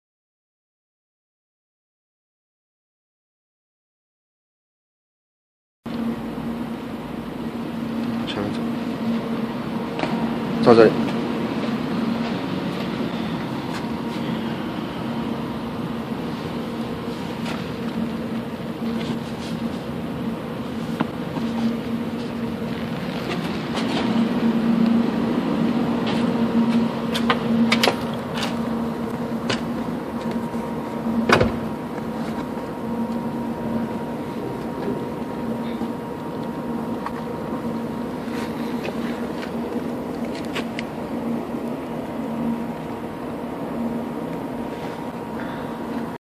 Extension cable for Touareg sound 2
This video will show you how to install the extension cable for Touareg with sound 2, Please to get more information.